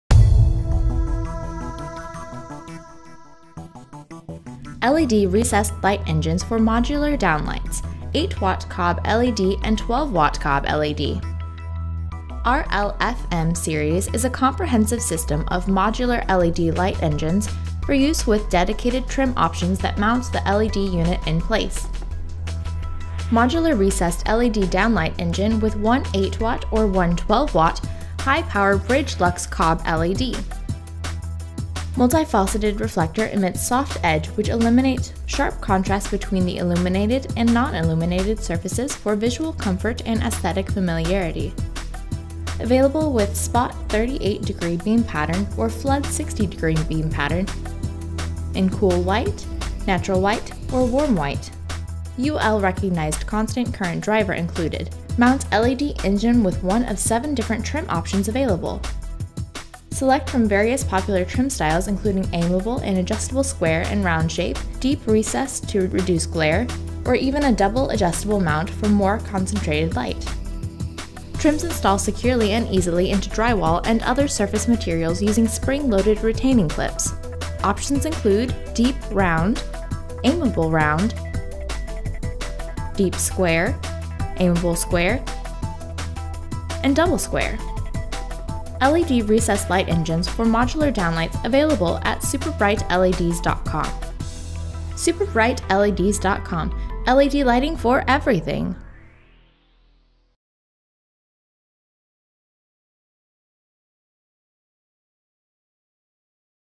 LED Recessed Light Engines for Modular Downlights
RLFM series is a comprehensive system of modular LED light engines for use with dedicated trim options that mount the LED unit in place. Modular Recessed LED Downlight Engine with 1 x 12 Watt or 1 X 8 Watt High Power Bridgelux COB (Chips-on-board) LED. Mount LED Engine with 1 of 7 different trim options available. Multifaceted reflector emits soft edge which eliminates sharp contrast between the illuminated and non-illuminated surfaces for visual comfort and aesthetic familiarity. Available with spot 38 degree beam pattern or flood 60 degree beam patterns in Cool White, Natural White, or Warm White. UL recognized constant current driver included, 100~240V AC operation.